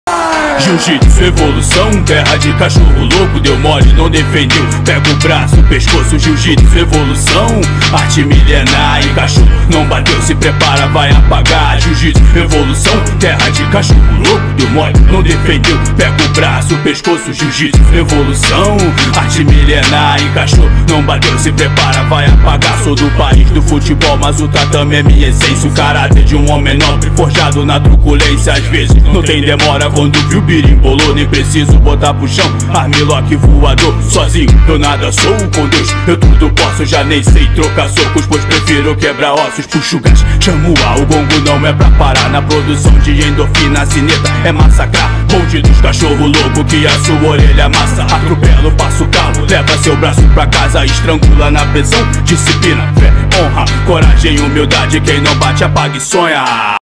Jiu Jitsu Evolução
Mais um dia de treino concluído graças a Deus!
Obrigado a toda nossa família Impacto Team @impactoteamaraguaina
No treino de hoje: @ricardims1 @duda_noleto_eduardo @valdijorjunior @sergio_assuncao @wylian_gabriel @felipeduardo_judo
OSS